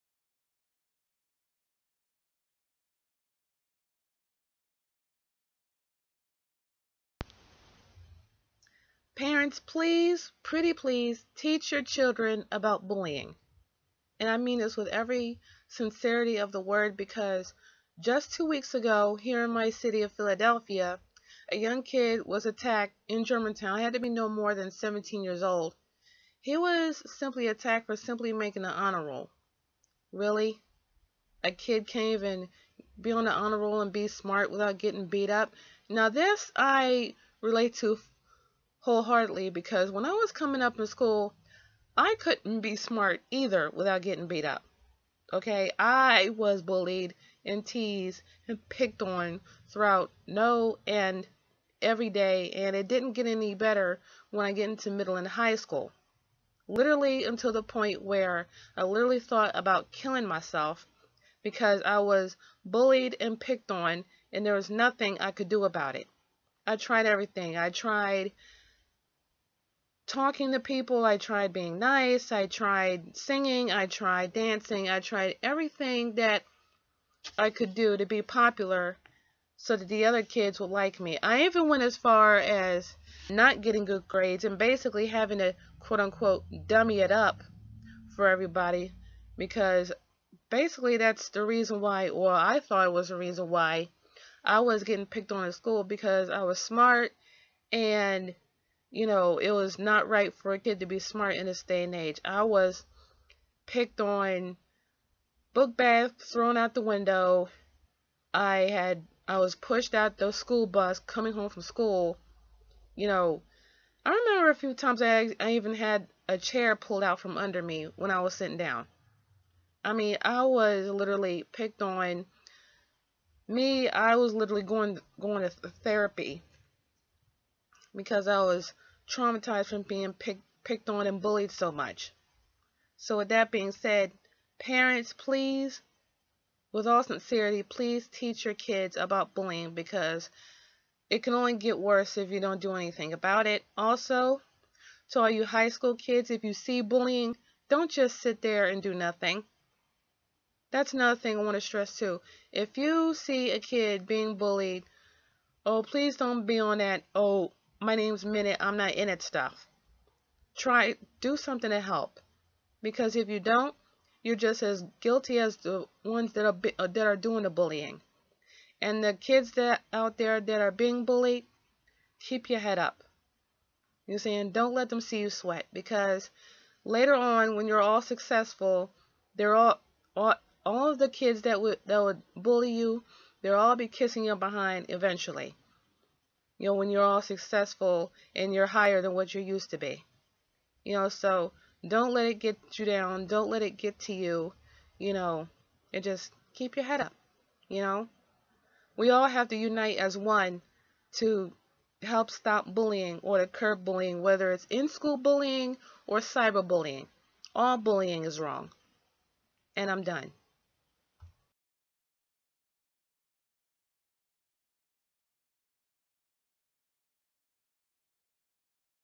Parents Please Teach Your Kids about
- I can't stress this enough, I know all too well how it feels to be bullied and picked on. and for being smart no less! There was a young boy here in Philly that got beat up by a bunch of other boys because he made the honor roll...REALLY? I was picked on for that and I wasn't pretty like the other girls on top of that so I know how that feels.

- Parents please teach your kids about bullying, kids who see other kids get bullied do something to help because if you don't you're just as bad as the kids doing the bullying. And to all the kids out there being bullied, keep your heads up. It will only get better in the end.